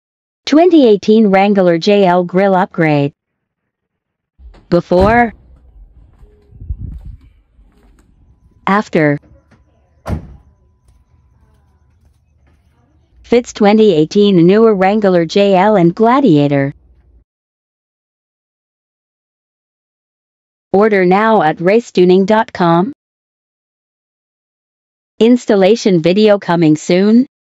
Jeep Wrangler Grille Inserts - Before & After - with Gladiator Grille Insert Conversion
We upgraded the grille on our 2018 Jeep Wrangler JL with Jeep Gladiator grille inserts. These grille inserts were initially made as replacements for the Gladiator, but they also fit the 2018-newer Wrangler JL. These are a nice cosmetic upgrade from the factory Wrangler JL inserts.

Fits Gladiator and 2018-newer Wrangler JL.

Order them below from our website.